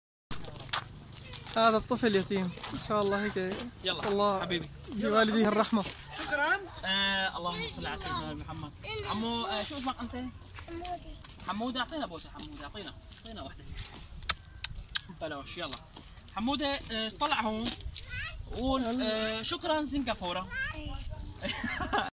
[An Orphan in the Refugee Camp] Warm Meals for Syrian Refugees
This is an orphan in the refugee camp. At least now we have a way to help them. May Allah accept our humble donations and forgive us for not being able to do more